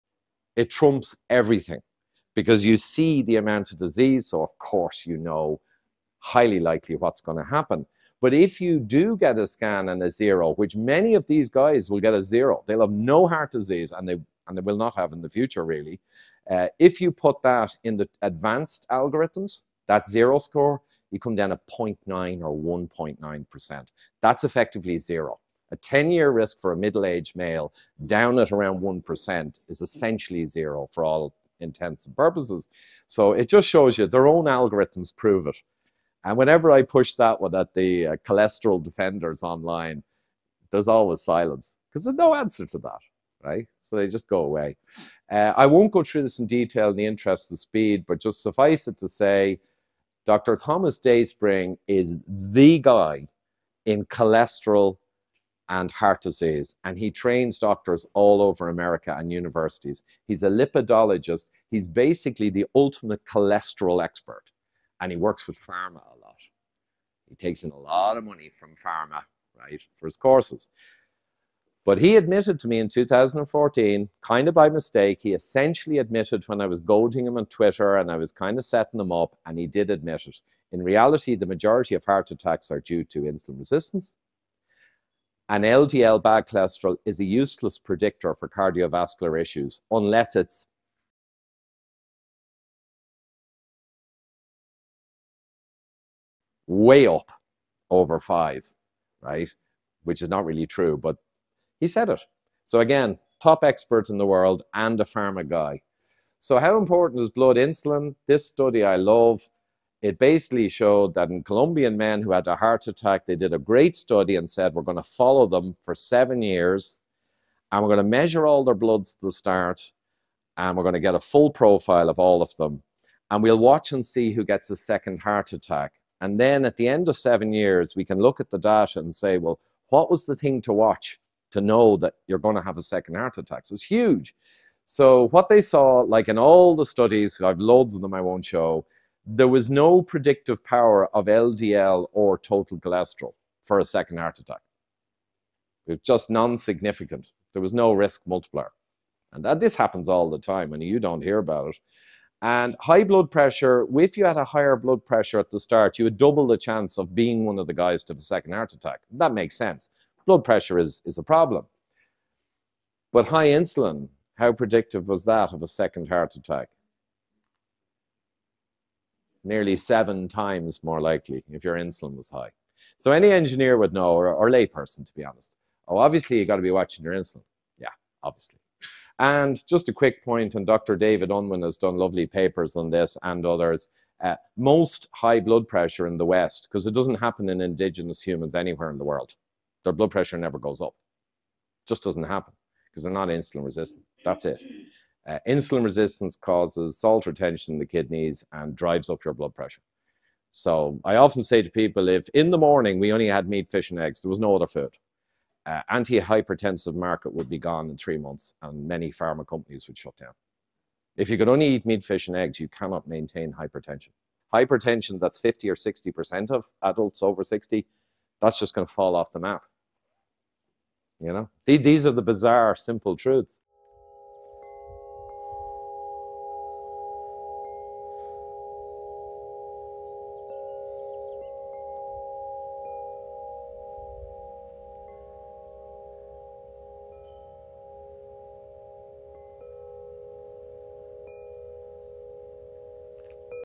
IVOR CUMMINS e | STUDY: Heart Attack risk? -LDL = 0 -high blood pressure = 1.9x -high INSULIN = 7X
presents episode 1535 | IVOR CUMMINS
Lowcarbskipton.com  podcast

If your CAC [coronary artery calcification scan] score is
zero...risk of heart attack is under 1% [essentially 0]
-medical establishment's own algorithms prove it!

Dr Thomas Dayspring admitted to Ivor:
"Majority of MI [myocardio infarctions] are explained by IR”
"LDL cholesterol is a terrible biomarker”

Columbian study: followed men who had heart attack,
for 7 years, measured blood labs at start and finish…
-What Was The Real
 Thing to WATCH??
Result: No Predictive
power for LDL or TC
for second heart attack!
-high BP = 1.9x risk
-high insulin = 7X

Dr David Unwin: high BP ONLY IN THE WEST…
-indigenous peoples have NONE
-IR causes salt retention
 in kidneys: drives up BP
"If we had ONLY fish,
 eggs, meat...in 3 months,
 ALL HYPERTENSIVE
 PROBLEMS GO AWAY”
 -big pharma shuts down”
High BP just goes away